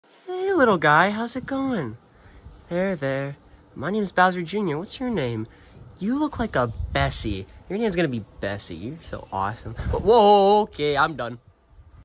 Bowser Jr and his Horse!🐴
Bowser JR and his new Friend Bessie the Horse!